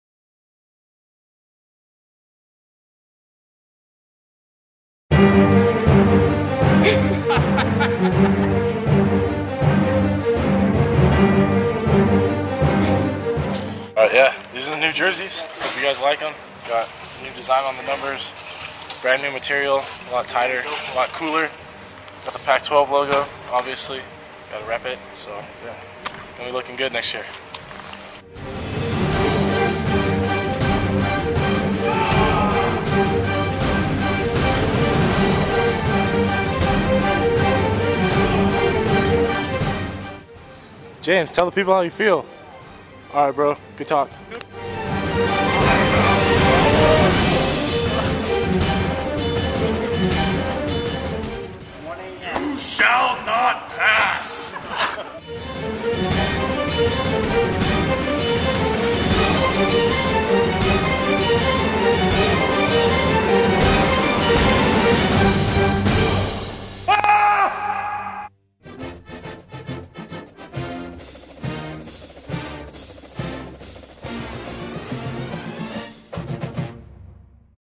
2011 Utah Football Intro Video -- Behind the Scenes
Check out the behind the scenes action of the 2011 Utah Football Intro Video.  Remember to be a Ute Walk two and half hours before every home game on 5th South!